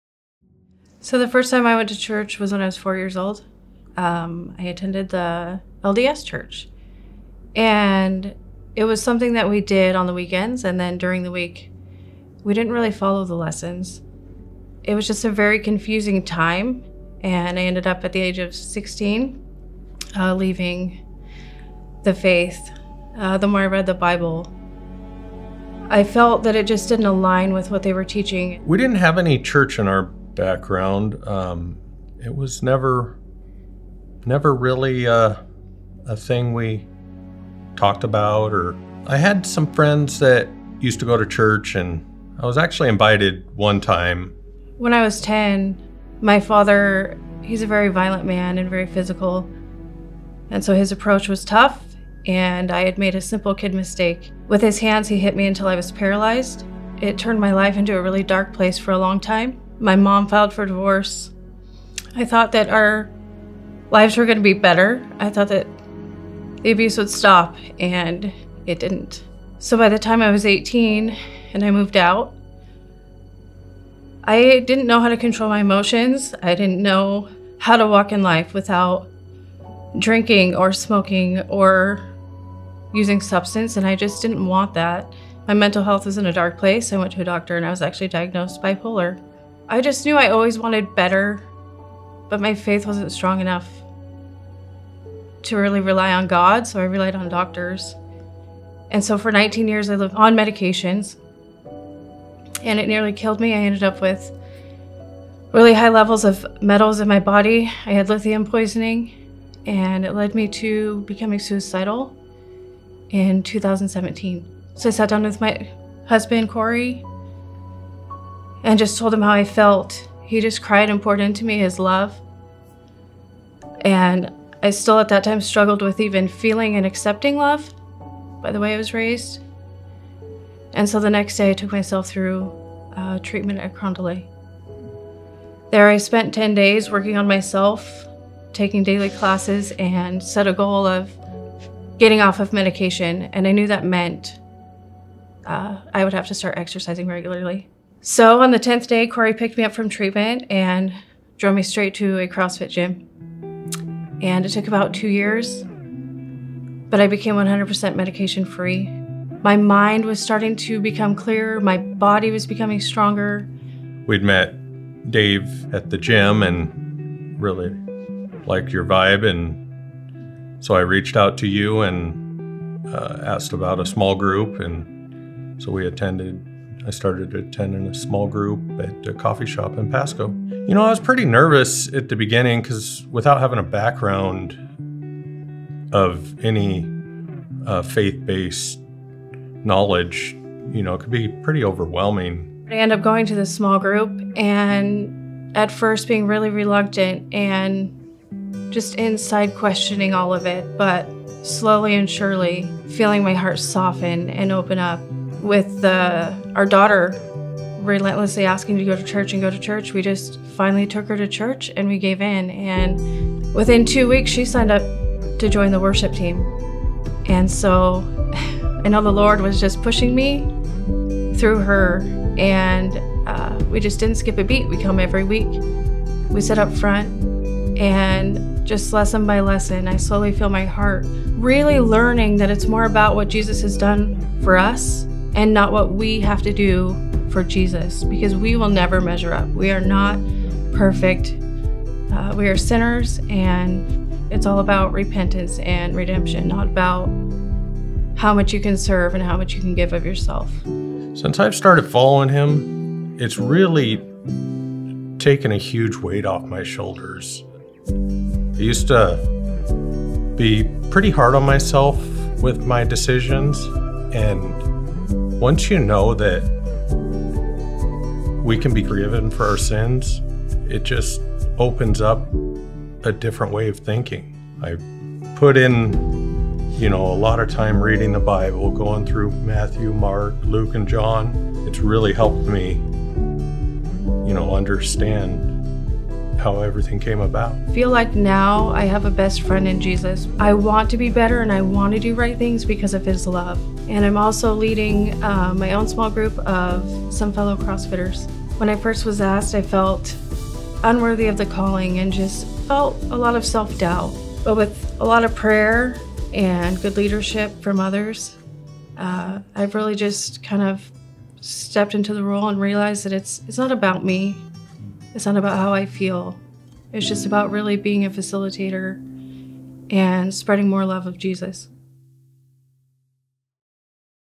Corey and Heather Finding Faith In Jesus - Transformation Story
This is the story of how Heather and Corey came to a faith in Jesus. We also celebrated with them as they were baptized!
“We come to church every week. Lesson by lesson I slowly feel my heart really learning that it’s more about what Jesus has done for us and not what we have to do for Jesus.” - Heather

Find out more at bethel.ch